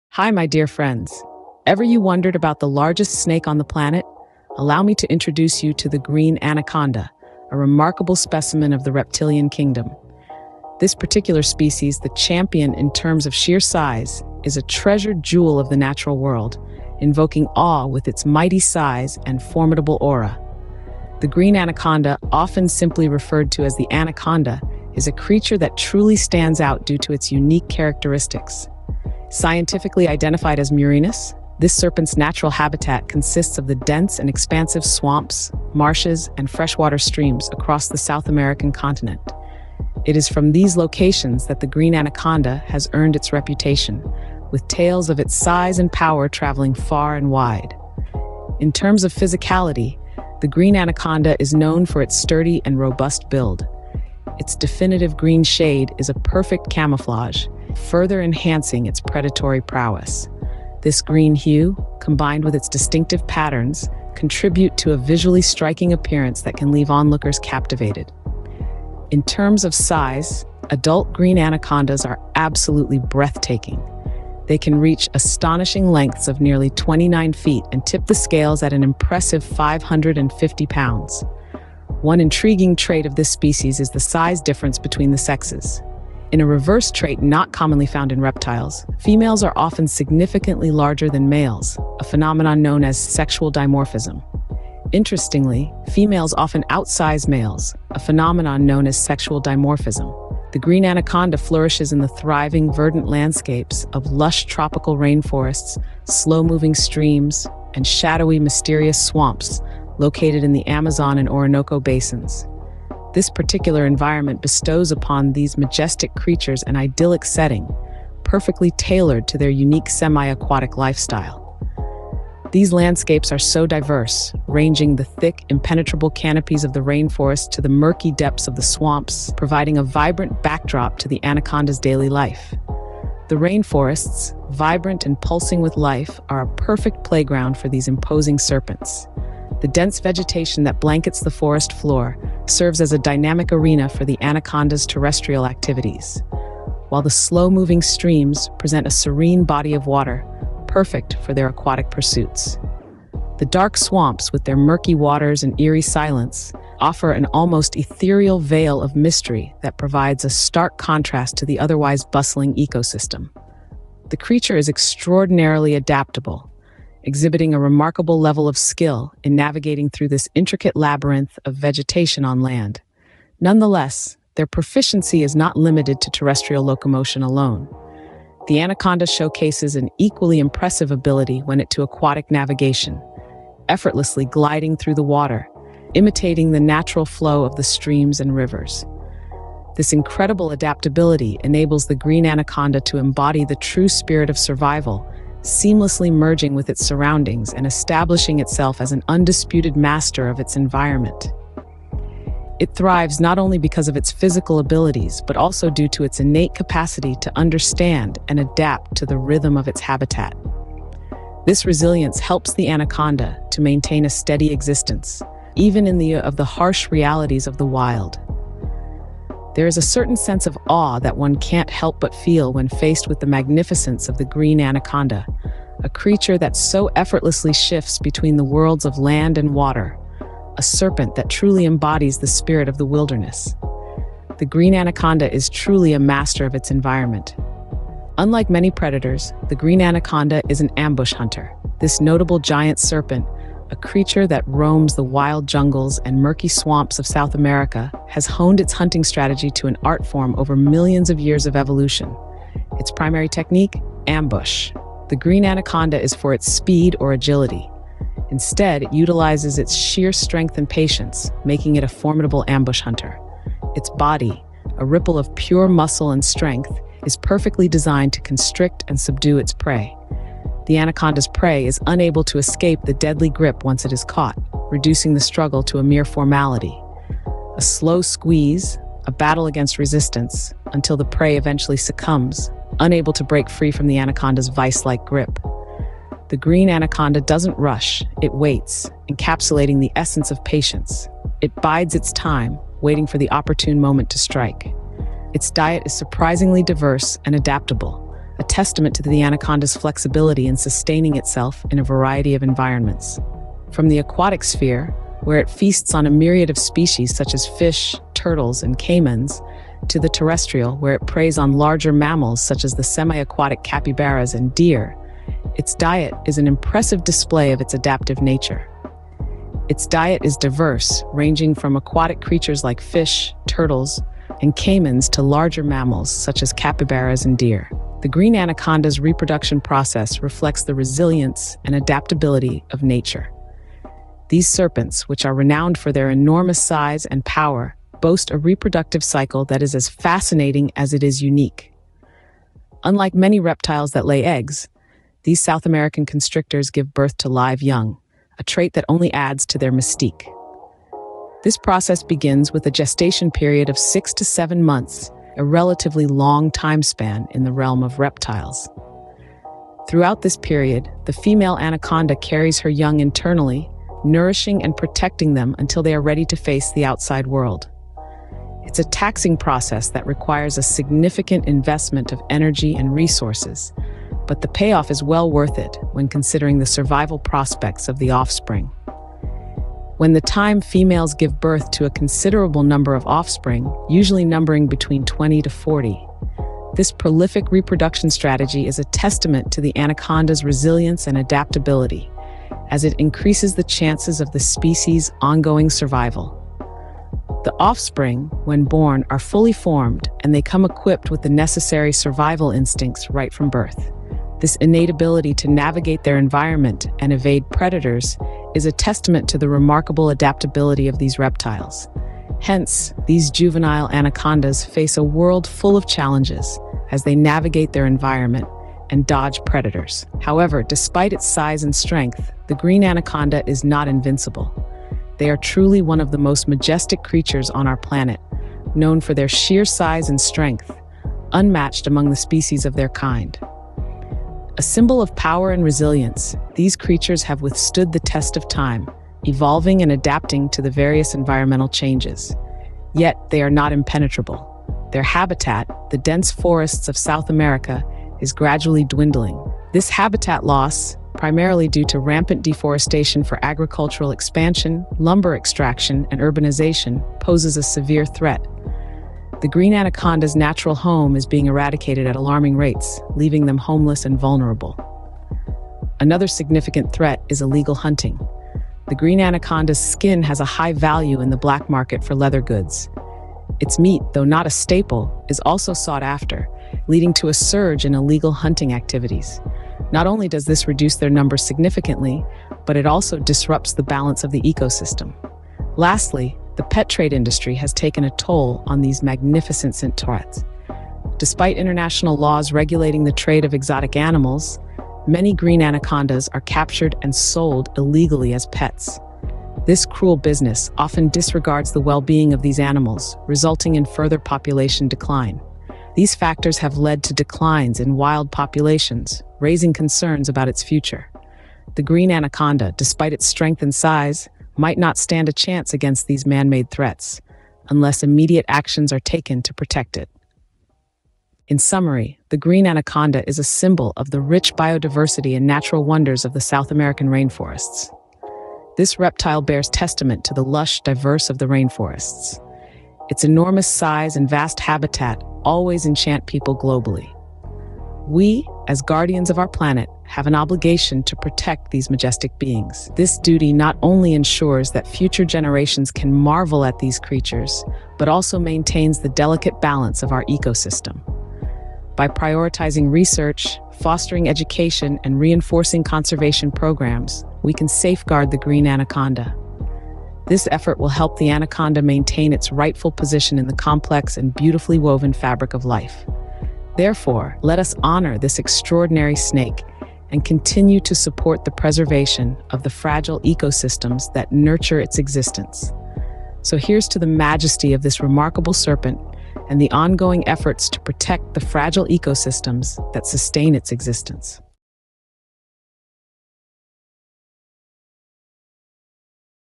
Giant Serpent: Green Anaconda
In this captivating video, we delve into the incredible world of the Green Anaconda, one of the largest and most powerful snakes on Earth. Join us as we uncover details about its habitat, unique characteristics, and the crucial conservation initiatives aimed at protecting this magnificent species.

Discover the secrets of the Green Anaconda's natural environment, its behavior, diet, and impressive hunting skills. Learn about the challenges these majestic creatures face in the wild and the importance of conservation efforts to ensure their survival for future generations.

From the swamps of South America to the complexities of its underwater hunting techniques, this video provides a comprehensive look at the awe-inspiring Green Anaconda. Don't miss out on this educational and eye-opening exploration!

OUTLINE:

00:00:00 Introduction to the Green Anaconda
00:01:40 Habitat and Lifestyle
00:03:58 Hunting and Diet
00:05:27 Reproduction and Survival
00:07:00 Threats and Conservation
00:08:41 Call to Action